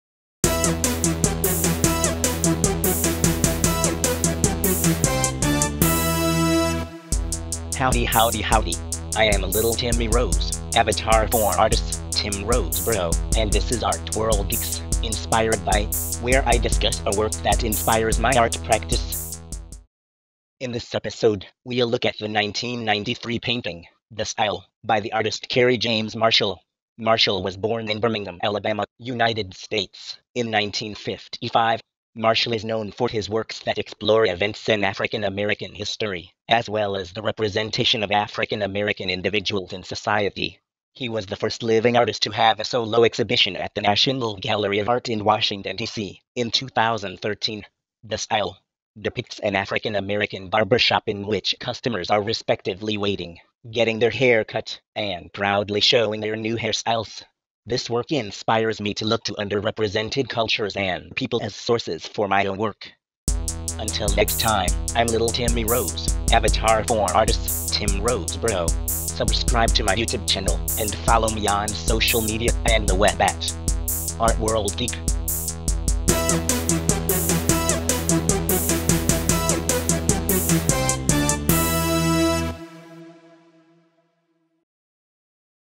ARTWORLDGEEK's INSPIRED BY: Kerry James Marshall
Lil' Timmy Rose looks at Kerry James Marshall

In "ARTWORLDGEEK's INSPIRED BY...," Lil' Timmy Rose (avatar for artist Tim Roseborough) discusses a work that inspires his art practice.

In this episode, we’ll look at the 1993 painting, “De Style,” By the artist Kerry James Marshall.